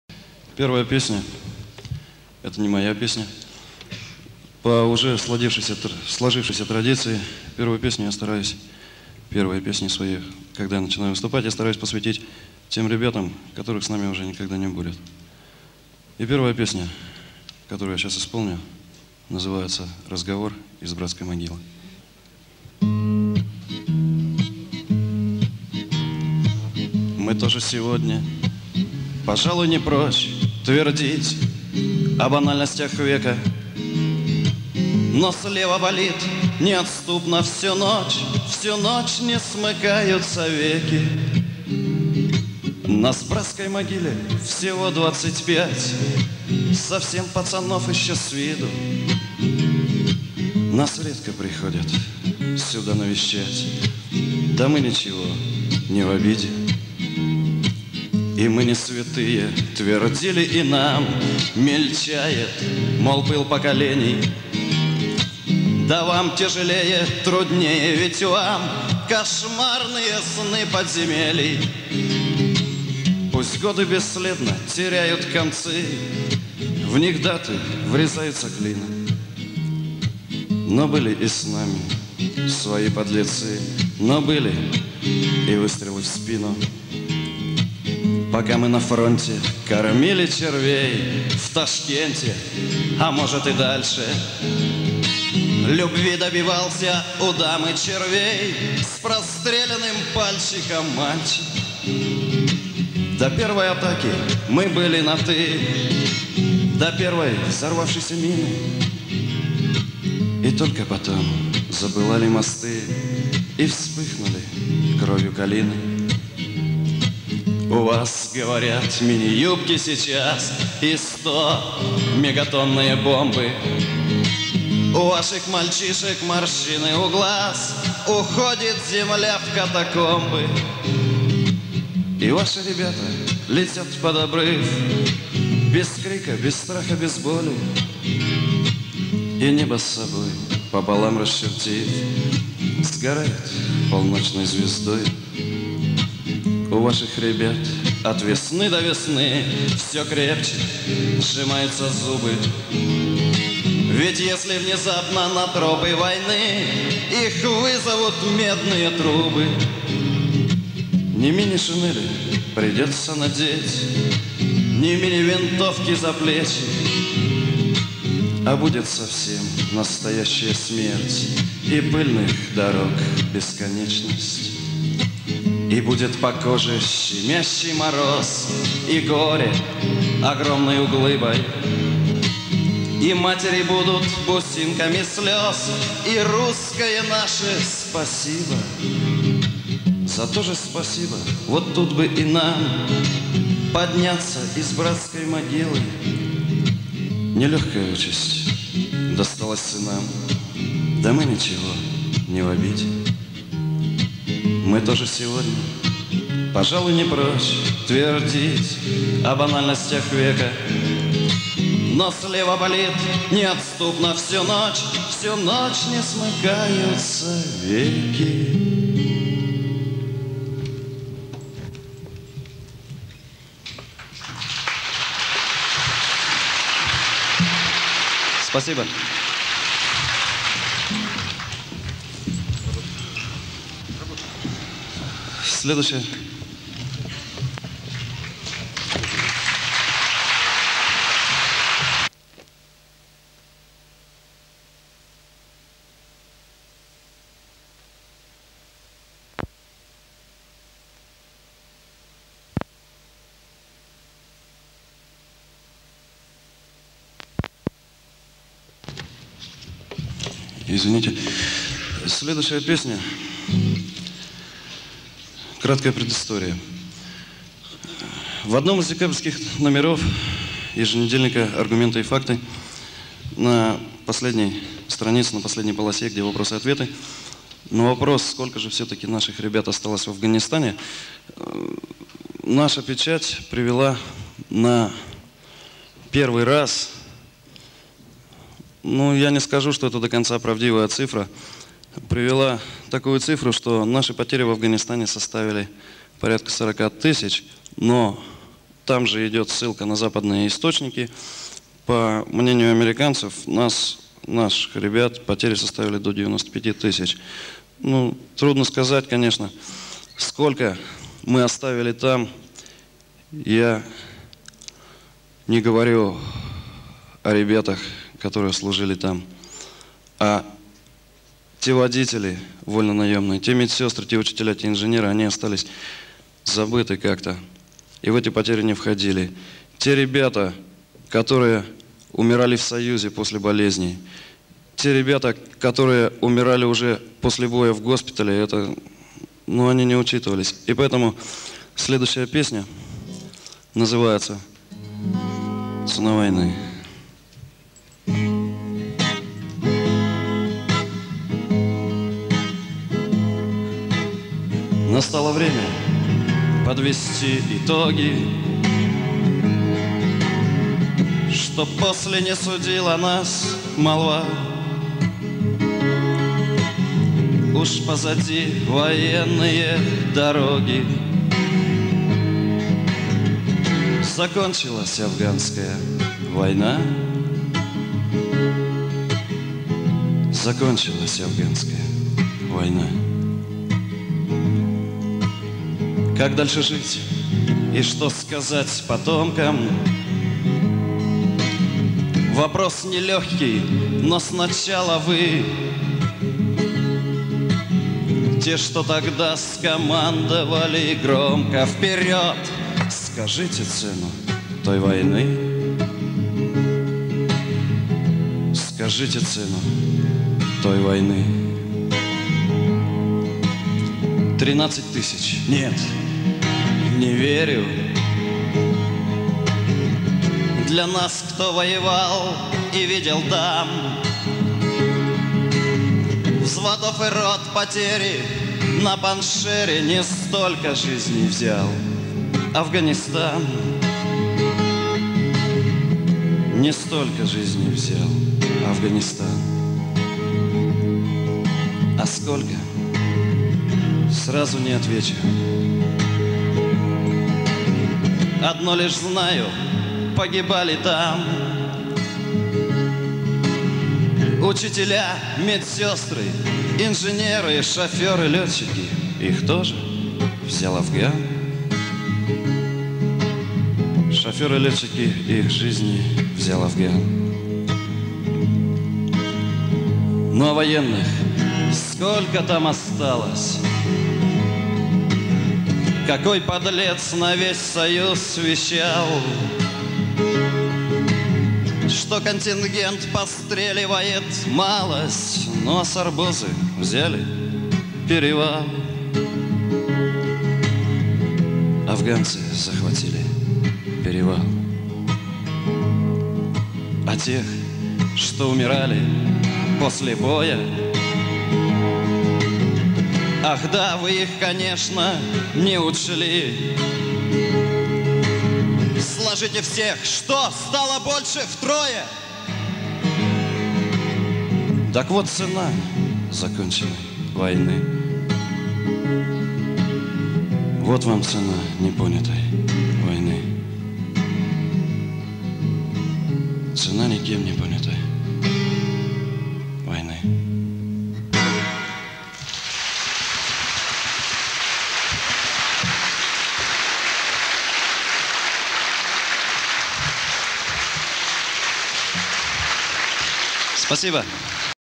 Вячеслав Тухтин - Ярославль 1990 г.
Фестиваль "афганской" песни Ярославль 1990 г.
Видео из архива Михаила Михайлова, оцифровка - Петр Трубецкой.